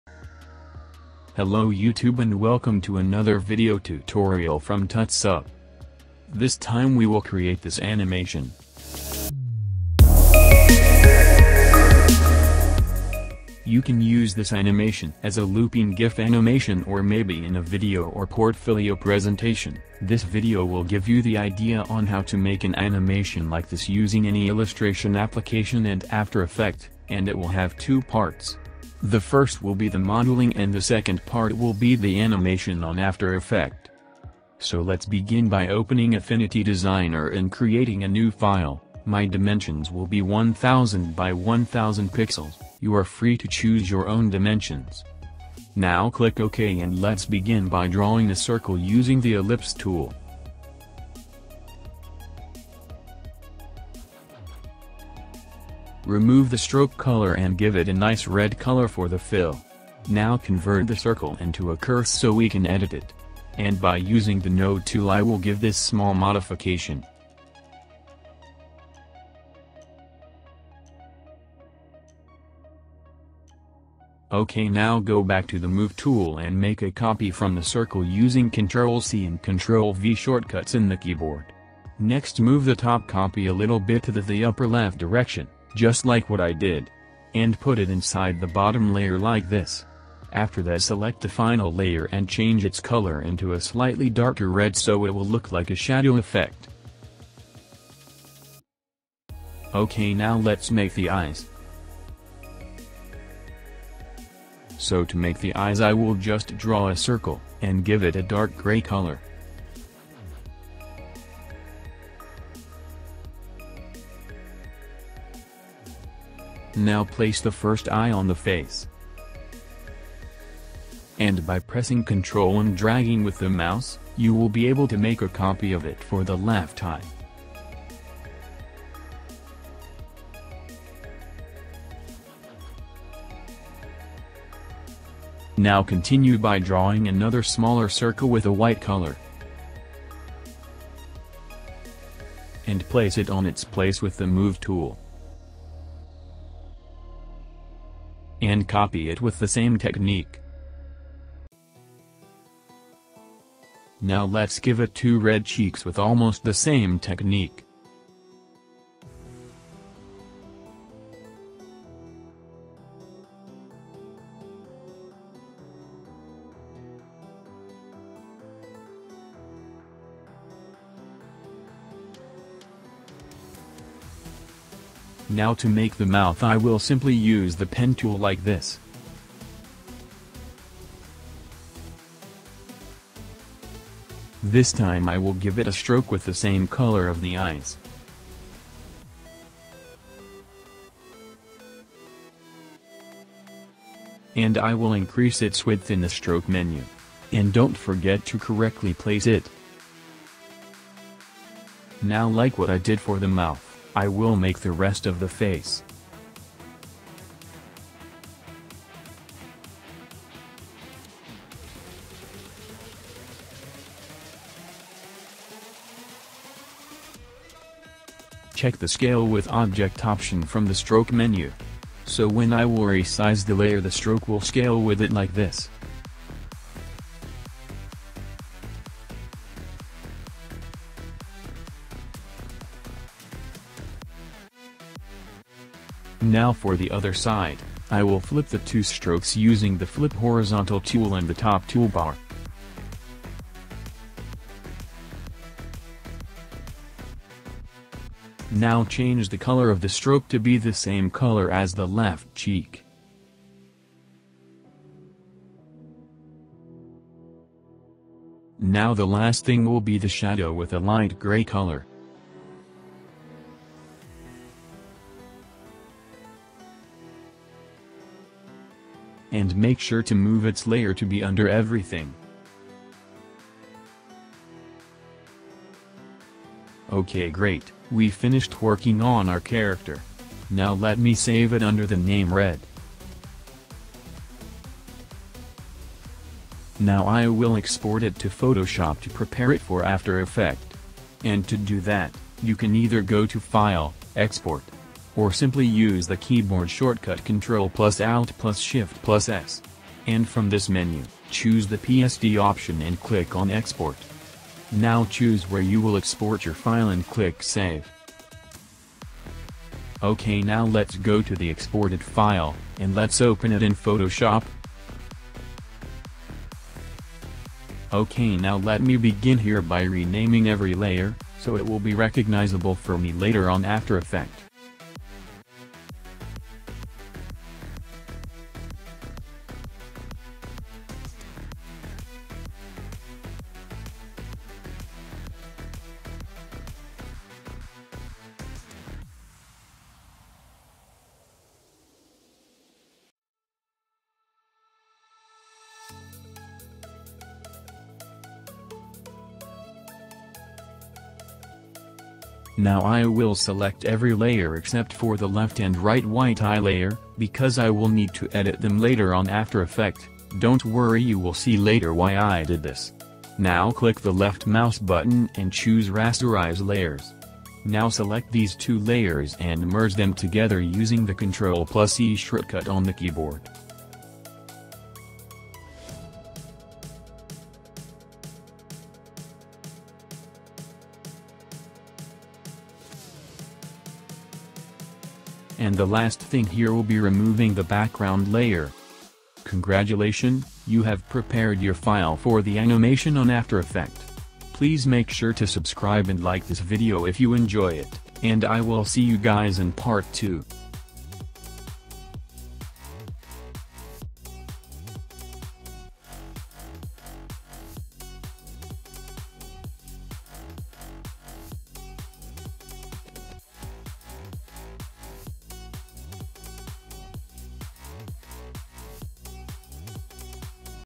Tutorial - Create a Cartoon Animation Using Affinity Designer and After Effects - Part 01 Tuto 02
Hello again :) this time i will show you How to Create a Cartoon animation Using Affinity Designer and After Effects

this video tutorial is the part 1 of 2 parts

in this video we will create the charracter on after effect and i will show you how to prepare it in photoshop for the animation in after effect